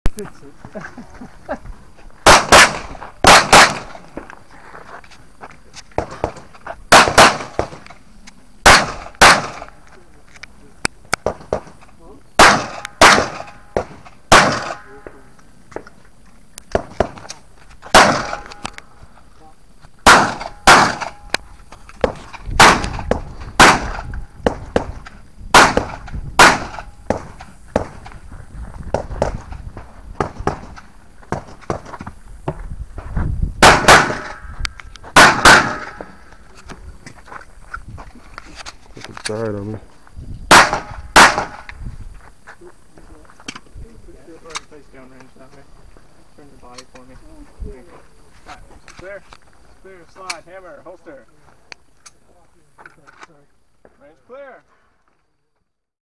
USPSA match shot in Duncan OK.
This is the fourth iteration shot on this day. This was the first pistol match in 30 months after the move to Oklahoma and major back surgery.